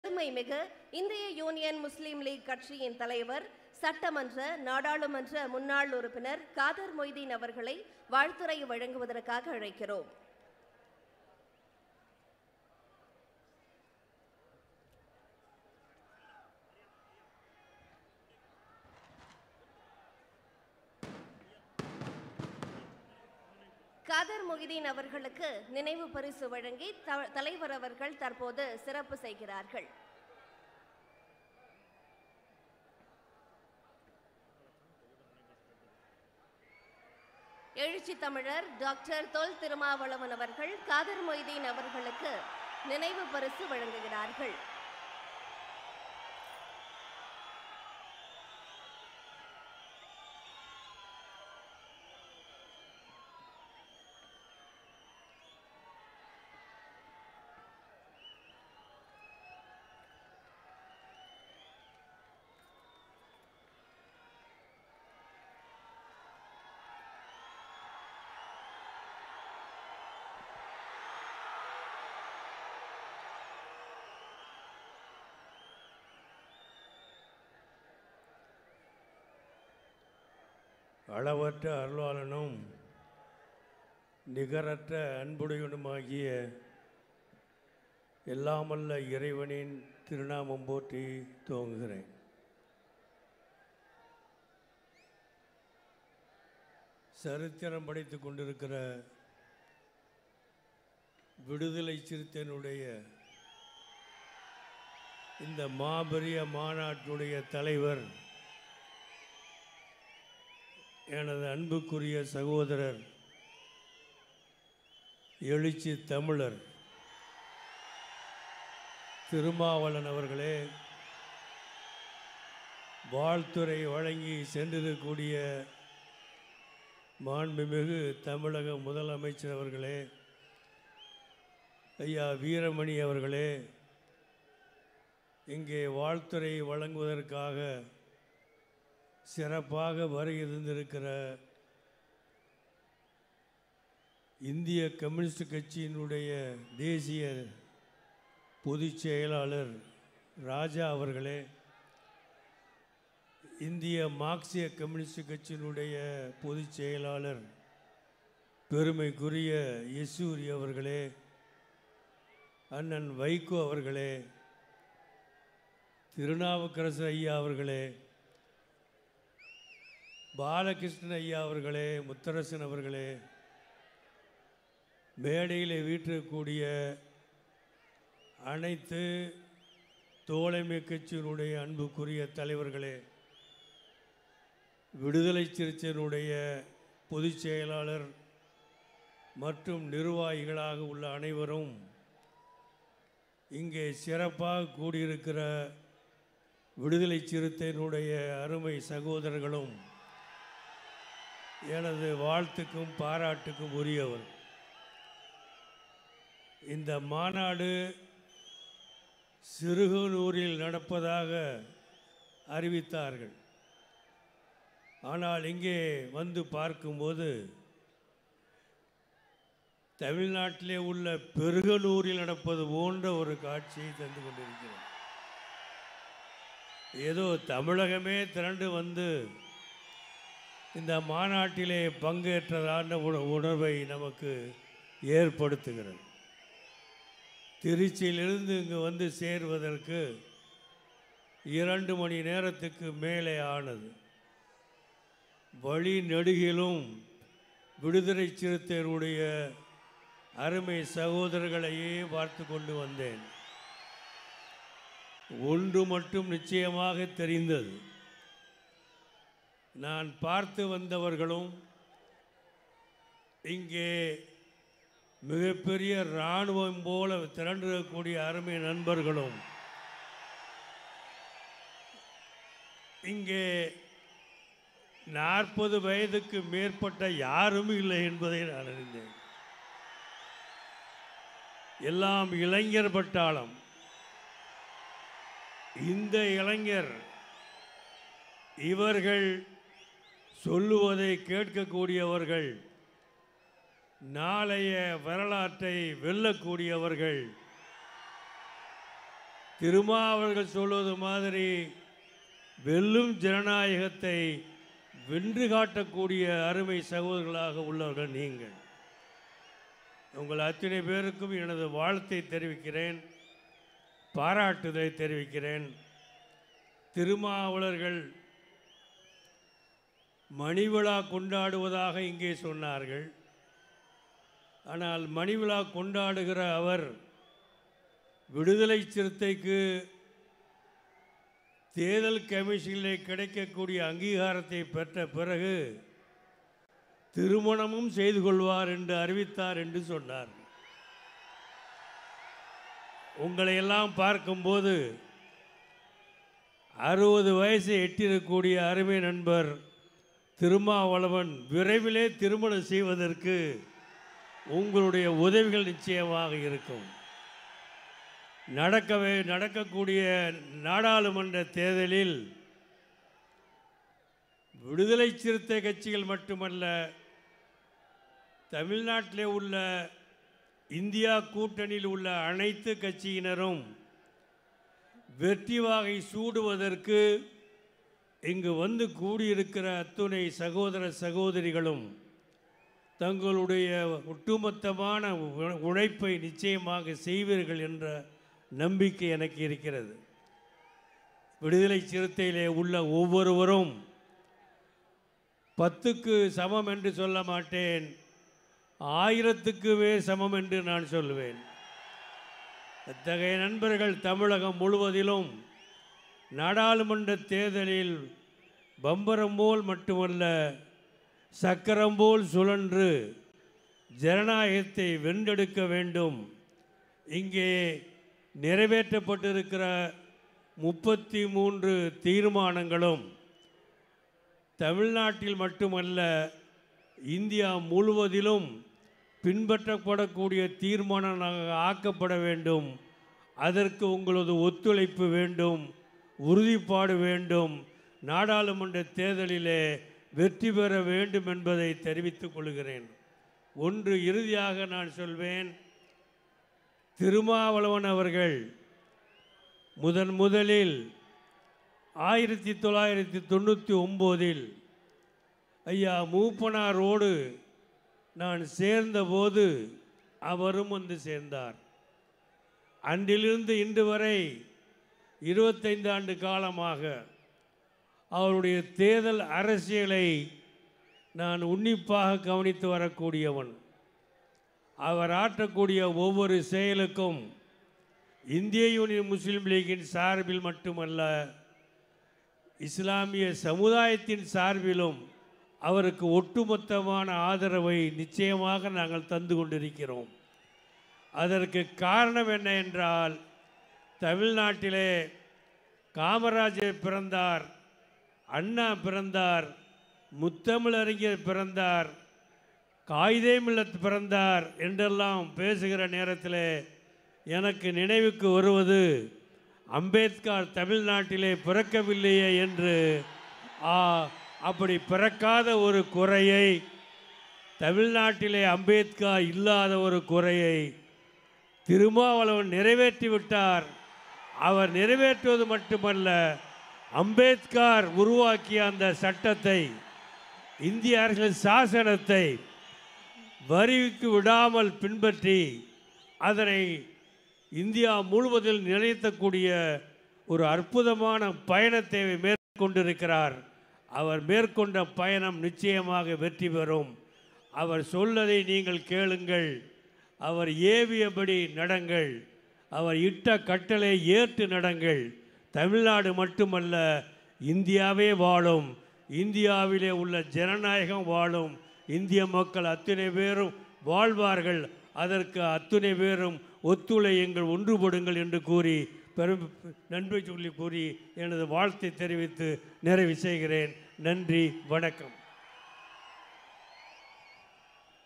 K. M. Kader Mohideen Speech VCK vellum Jananayagam manadu | Thirumavalavan | Trichy | DMK | STV
KM.Kader Mohideen Speech VCK vellum Jananayagam manadu | Thirumavalavan | Trichy | DMK | STV